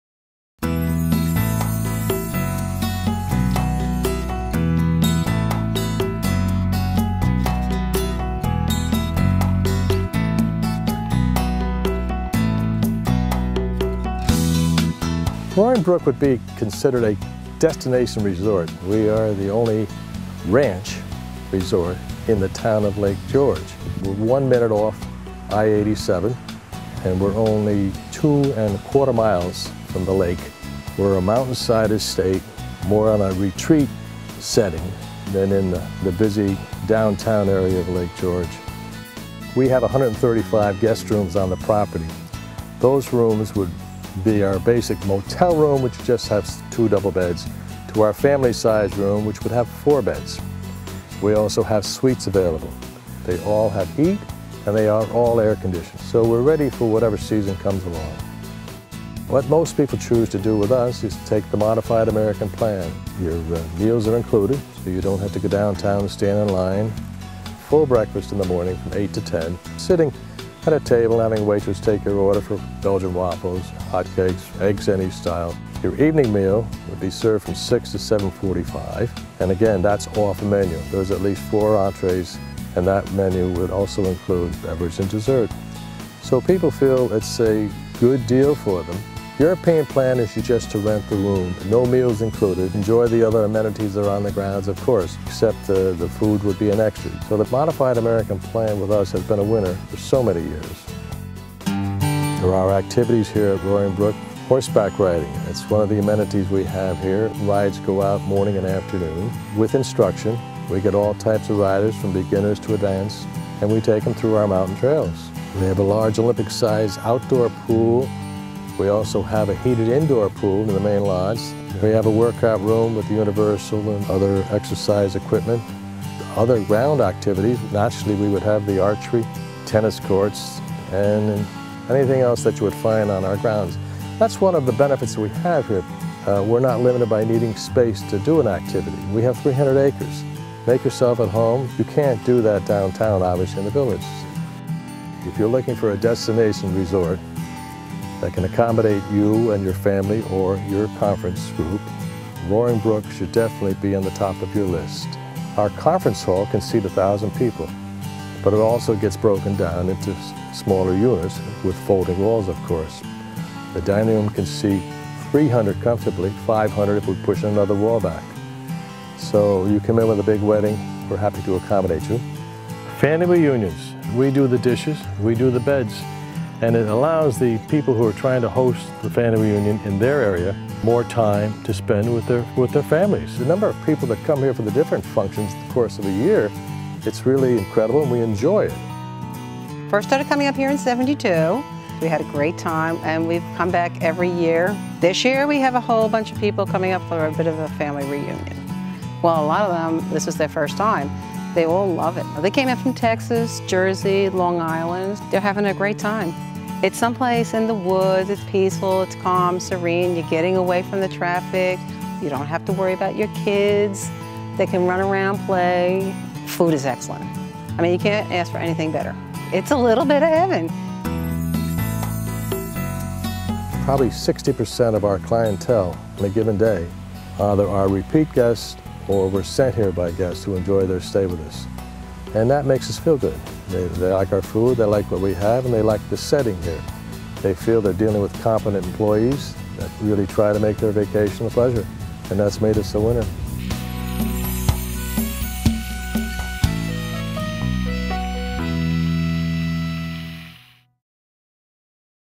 Roaring Brook Ranch Resort and Conference Center in Lake George, NY
Roaring Brook Ranch Resort & Conference Center is a popular spot for conferences and special events... from Americade to business functions. An all-inclusive pet-friendly family resort in Lake George with pools, three tennis courts, 25 horses, wilderness riding, saunas, fitness rooms & is near golf courses!